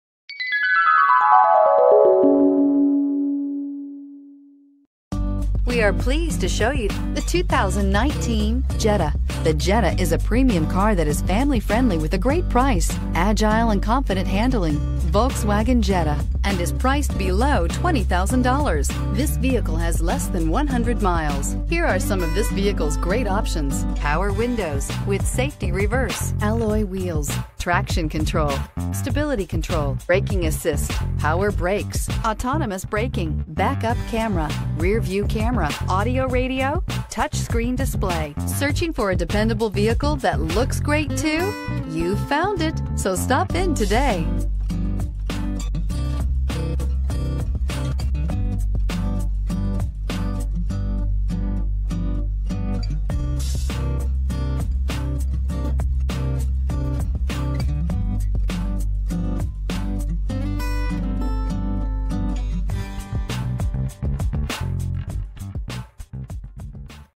2019 Volkswagen Jetta 1.4T SE NewNew or Used 10164
2019 Volkswagen Jetta 1.4T SE

When you're ready for an automotive upgrade, try this 2019 Volkswagen Jetta 1.4T SE on for size, which features a push button start, backup camera, blind spot sensors, Bluetooth, braking assist, dual climate control, hill start assist, stability control, traction control, and anti-lock brakes.  We're offering a great deal on this one at $19,945.  Drive away with an impeccable 5-star crash test rating and prepare yourself for any situation.  Drivers love the stunning black exterior with a titan black interior.  Interested? Call today to take this vehicle for a spin!